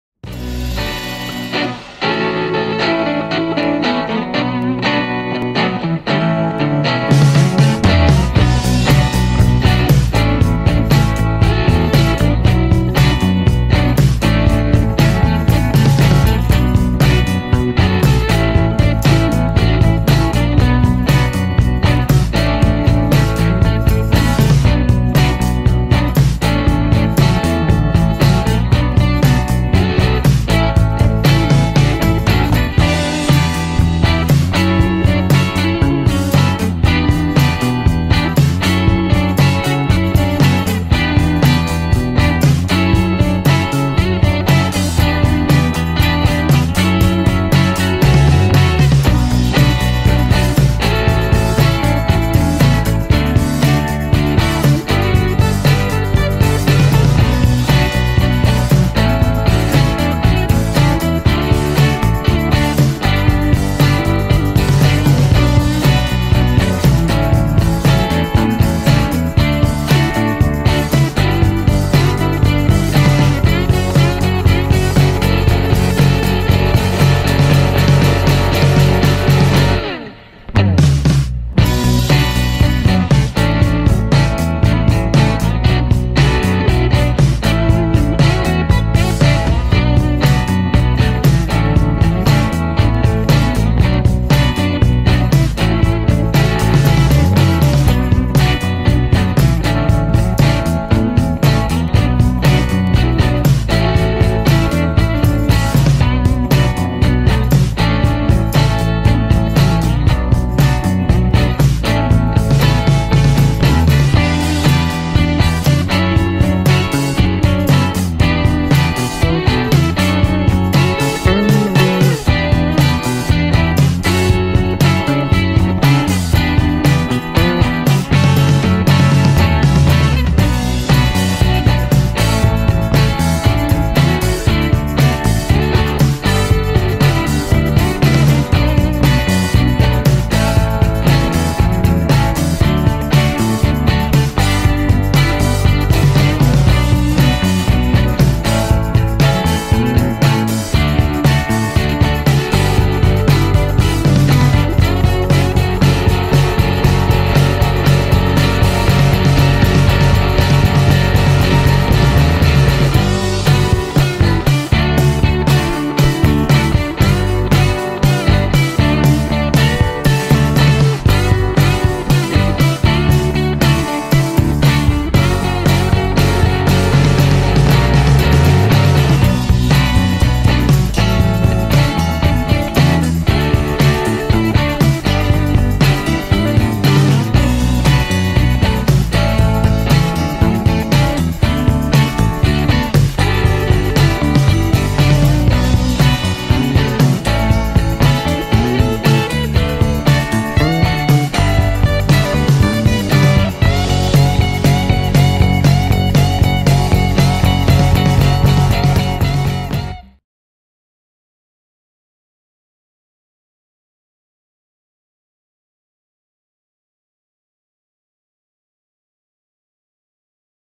Ponte General Manuel Belgrano - Chaco à Corrientes, Argentina
Passando pela ponte General Manuel Belgrano vindo do estado do Chaco pela Ruta 16 em direção ao estado de Corrientes e à Ruta 12.